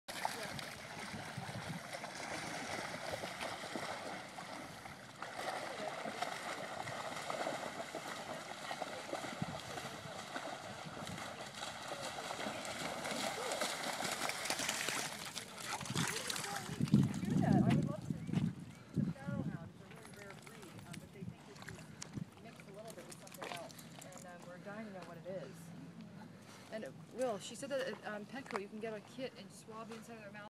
Dante's first swim
Dante's first time in water at Callahan park.